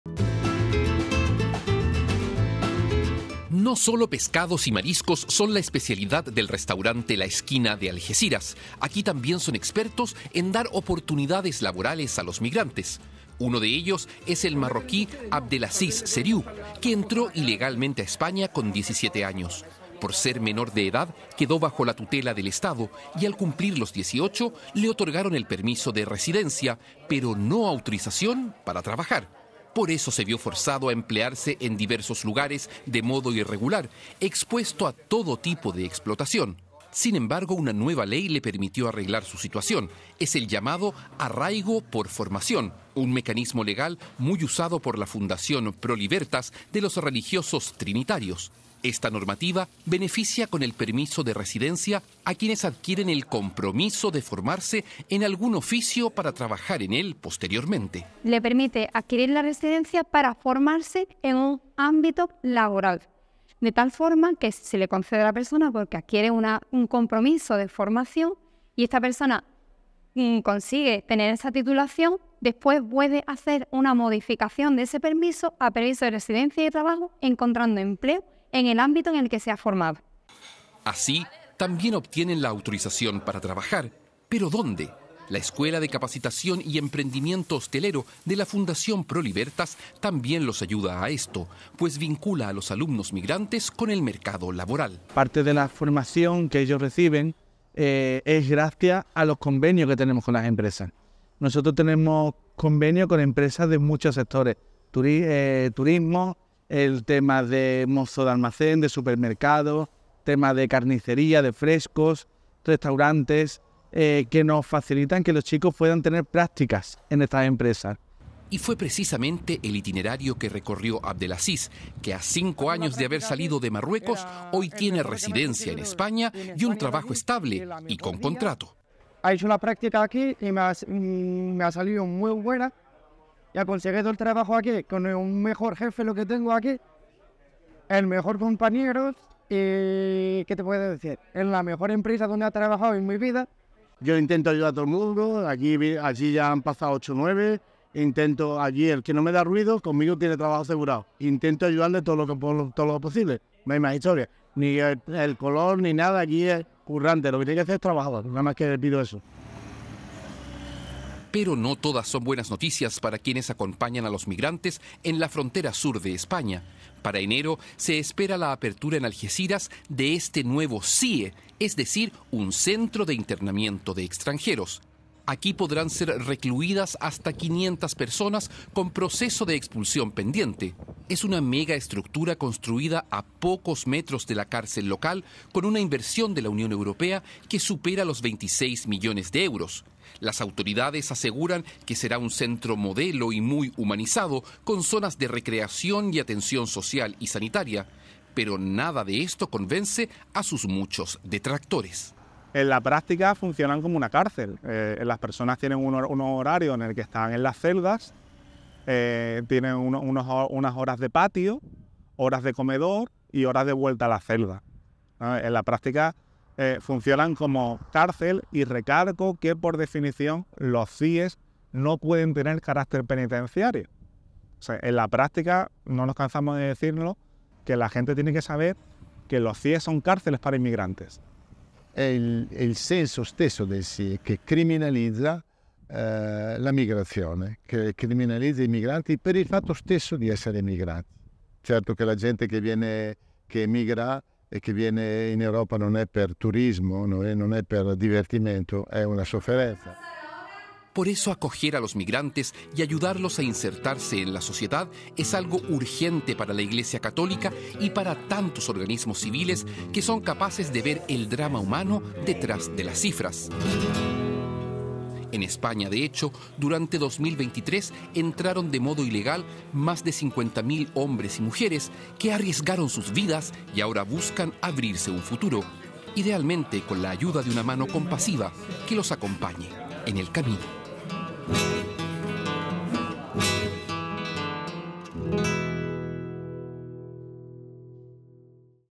Threats and opportunities for migrants on Spain’s southern border
While new legislation simplifies the issuing of residence permits for those embarking on formation for work, an enormous Detention Center for undocumented foreigners is under construction in Algeciras. Civil and Church organizations warn that this infrastructure could become a prison for individuals who have committed no crimes.